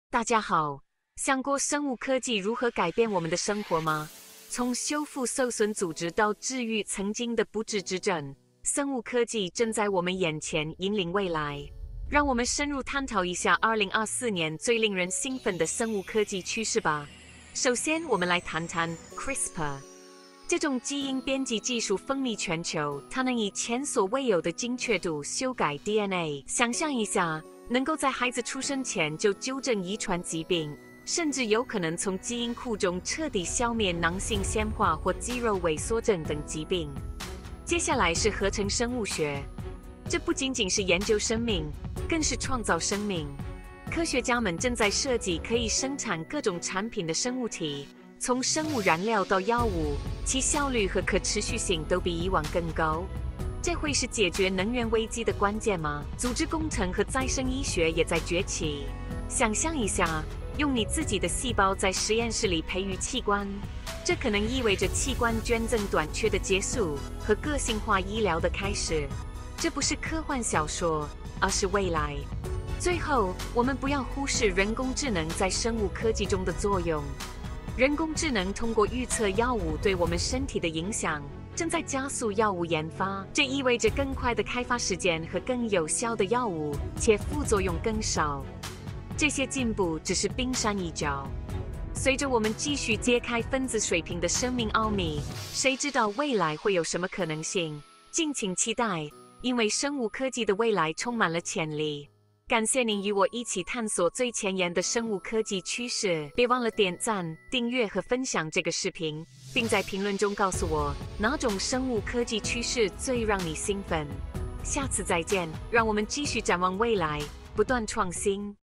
未来的科技：探索生物技术的最新趋势 (2 Minutes)
在这个启发性视频中，我们将深入探讨未来科技中生物技术的最新趋势。探索生物技术领域的创新应用，了解生物技术如何引领科技发展的未来。从基因编辑到生物医学，生物技术正不断推动科技的边界。加入我们，了解生物技术的最新趋势，探索其在科技创新中的重要性。